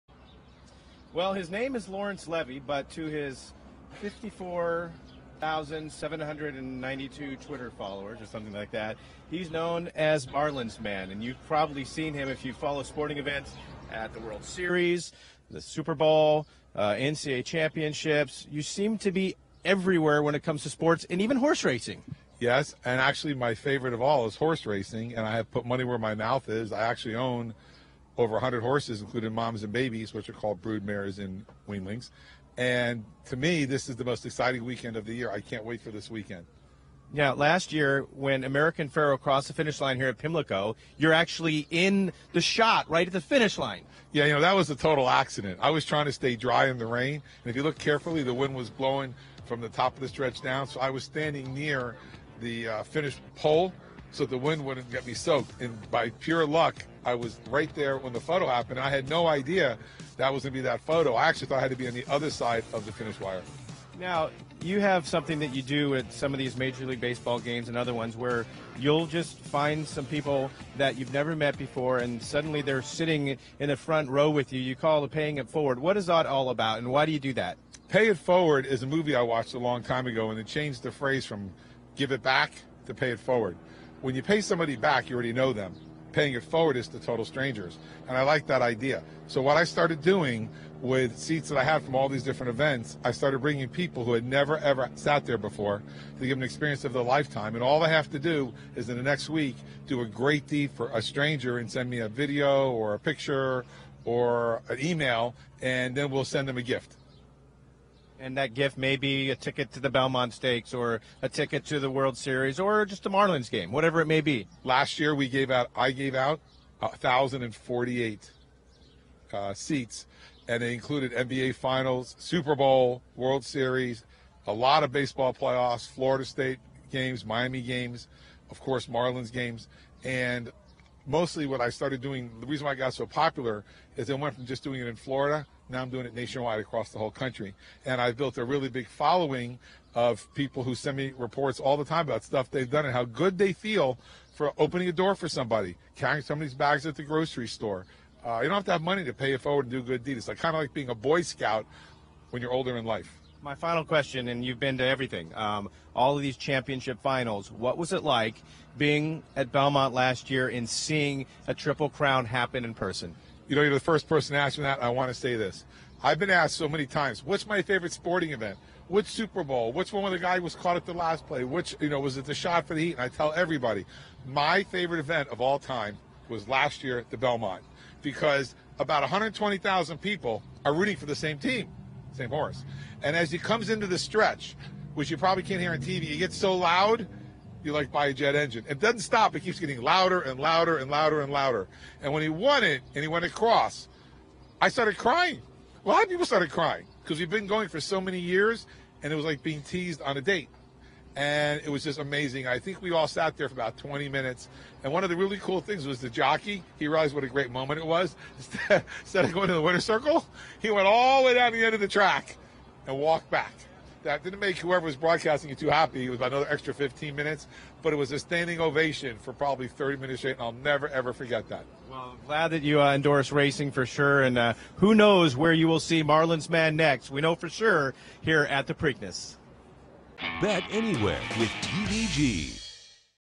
"Marlins Man" Enjoys Preakness Week
Dave Weaver caught up with the man known affectionately as "Marlins Man" for a wide-ranging interview, with topics ranging from horse racing to his concept of paying it forward.